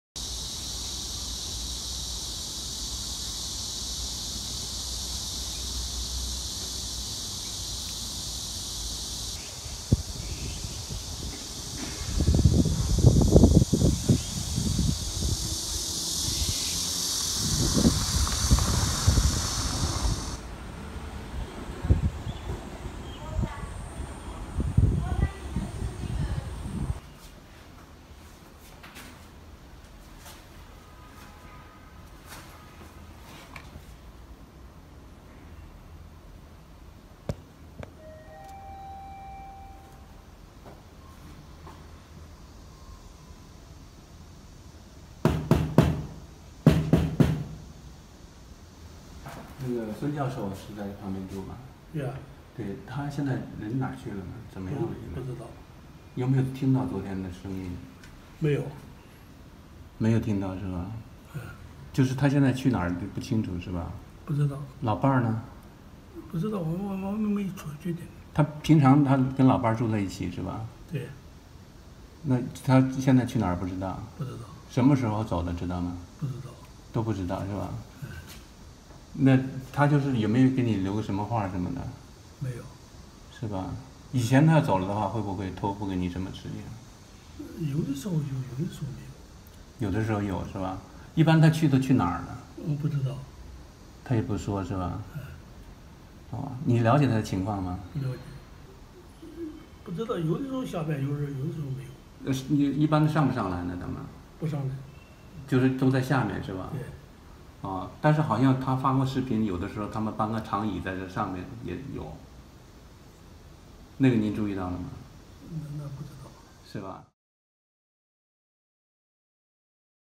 VOA Reporter Visits Home of Detained Professor Wenguang Sun
Professor Wenguang Sun is believed to be detained at Yanzi Mountain Villa, a military-run hotel in Jinan, eastern China. The professor was arrested Wednesday morning while speaking to VOA about his criticisms of China’s government. A VOA reporter visited Yanzi Mountain Villa, and the professor’s home on Friday, where neighbors said police have been monitoring him closely for a long time.